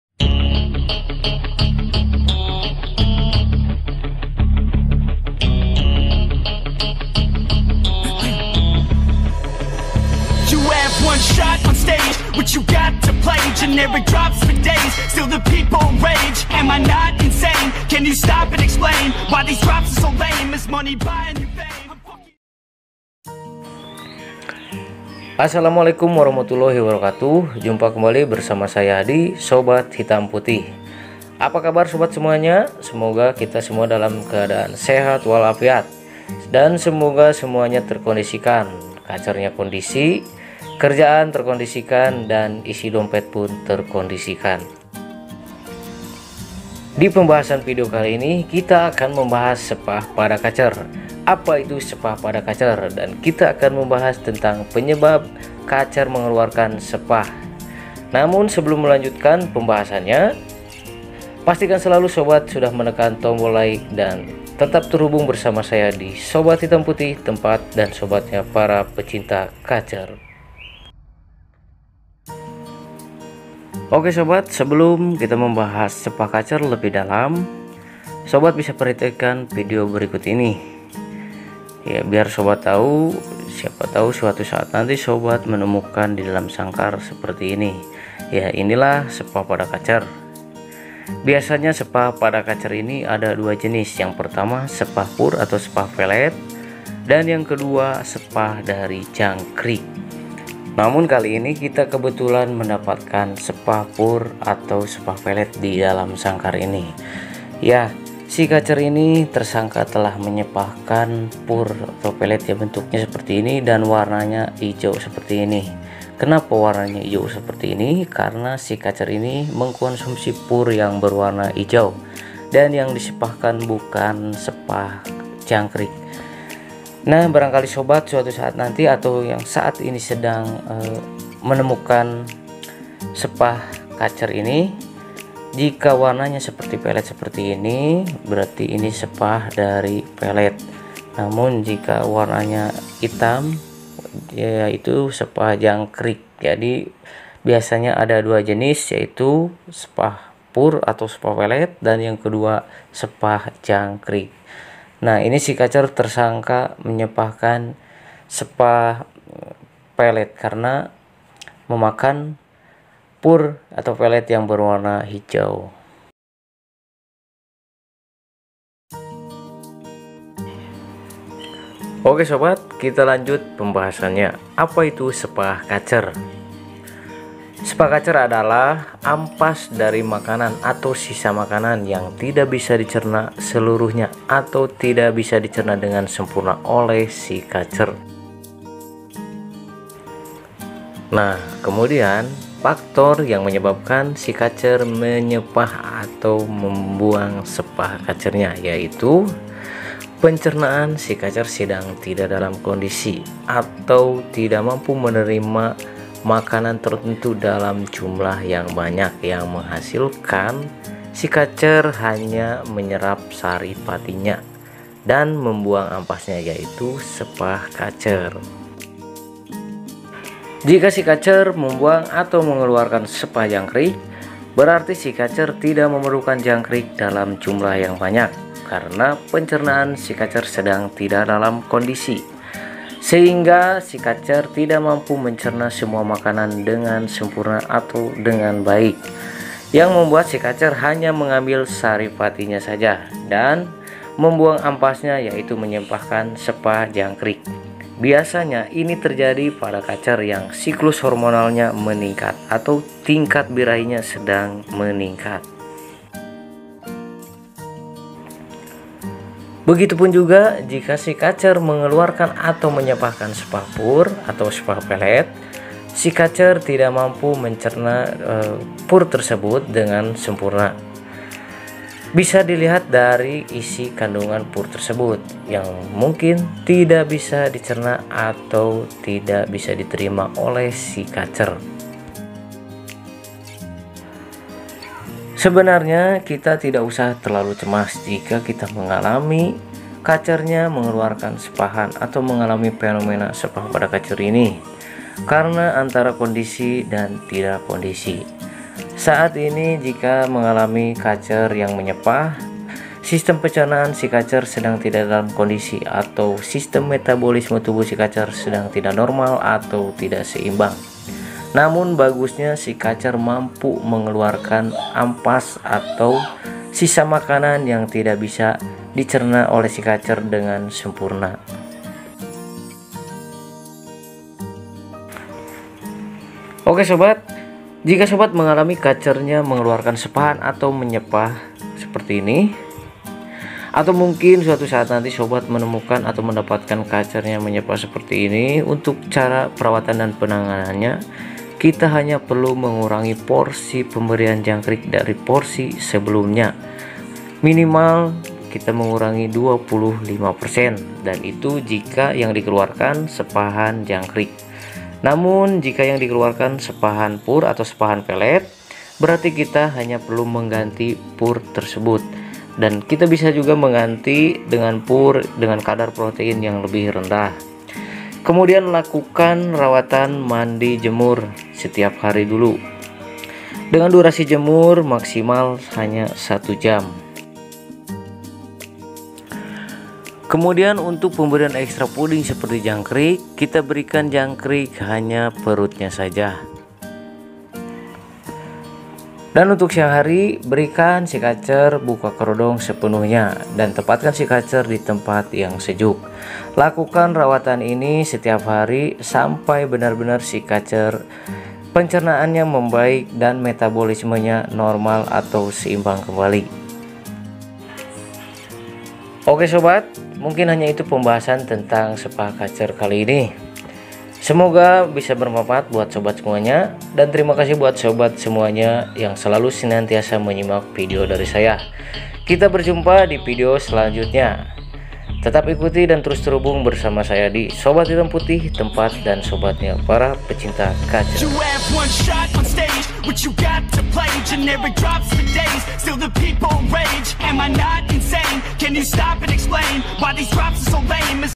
KACER MENGELUARKAN JIMAT !!Penyebab kacer nyepah dan cara perawatannya
Apa itu sepah kacer ?
Apa penyebab kacer nyepah dan bagaimana cara perawatannya?
mari temukan pembahasannya di video ini, supaya sobat tau apa itu sepah kacer, penyebabnya dan bagaimana cara mengatasinya apabila suatu saat nanti sobat mengalami kacer nya mengeluarkan sepahan.